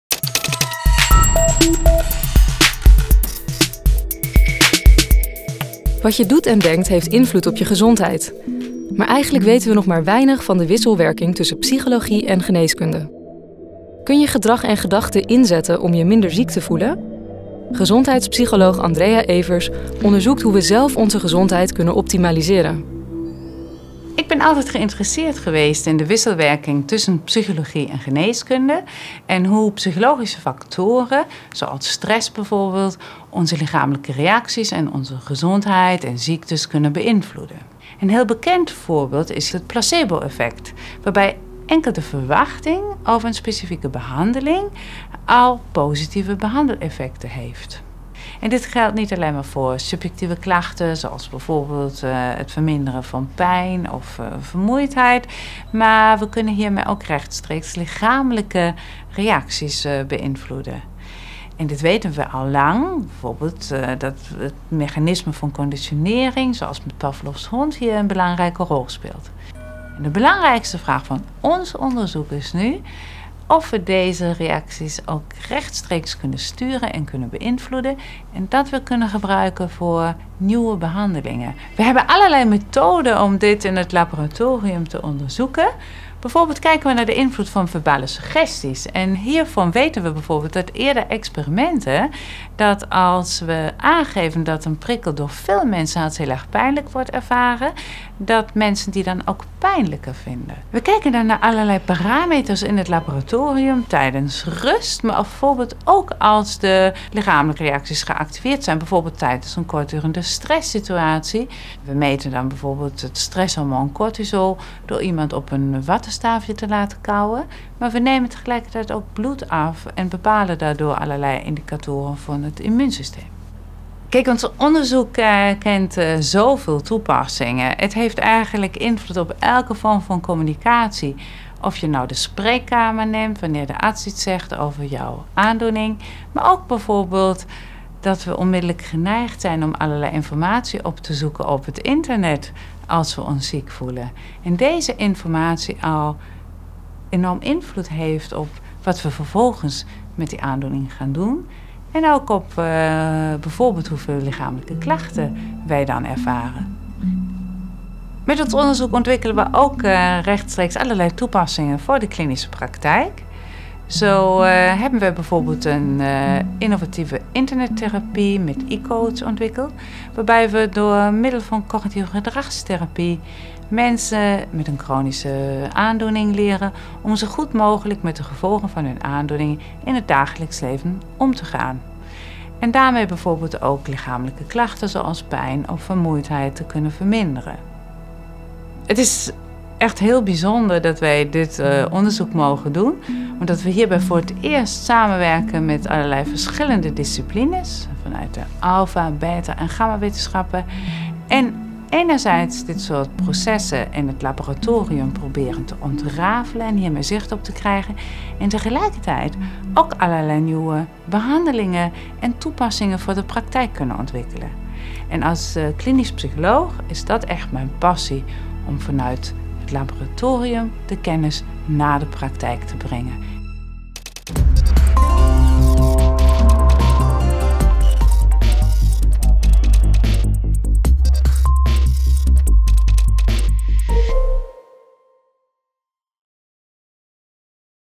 Optimise your health
Your actions and thoughts affect your health. But we actually still know very little about the interaction between psychology and medicine. Is it possible to use your behaviour and thoughts to feel less ill? Health psychologist Andrea Evers investigates how we can optimize our own health.

Made by: Aline Idzerda 2014
In cooperation with:
Graphics: SproetS